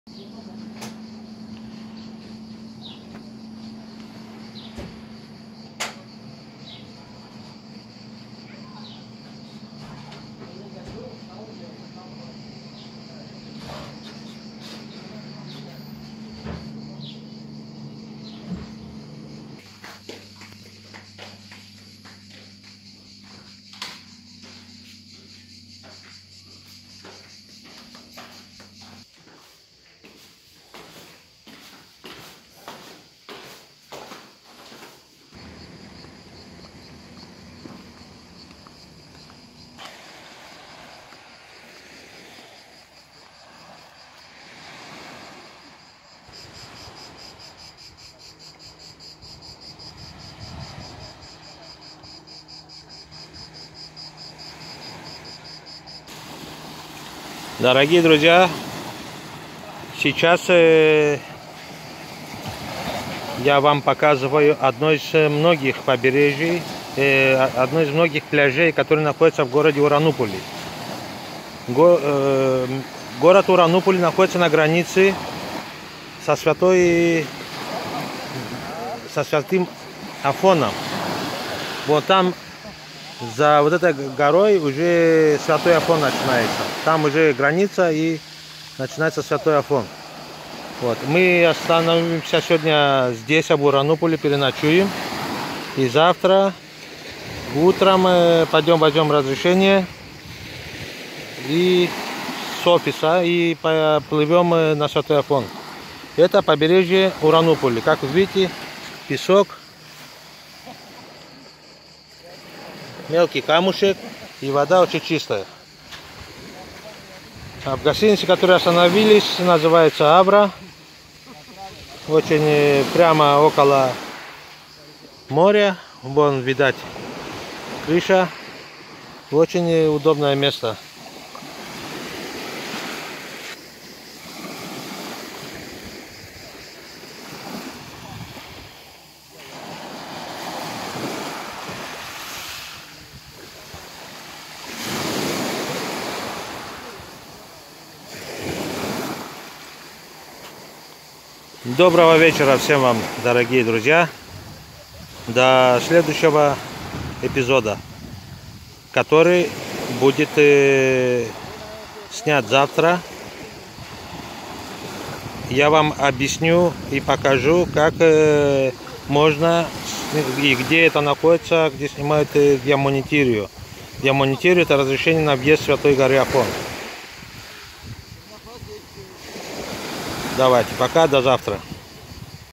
Приехали в Урануполи - порт, из которого отправляется корабль на Афон!
Место от которого отправляется корабль на Святую гору Афон называется Урануполи. Урануполи с греческого переводится Небесный город и находится в километре от границы монашеской Республики Афон, у основания третьего пальца полуострова Халкидики в 140 километрах к юго-востоку от города Салоники. В Урануполи находится порт и представительство монашеской Республики Афон, где выдается пропуск на Афон - Диамонтирион. Как попасть на Святую гору Афон я расскажу в следующих видео, а пока покажу Вам виды Урануполи из гостиницы, где мы заселились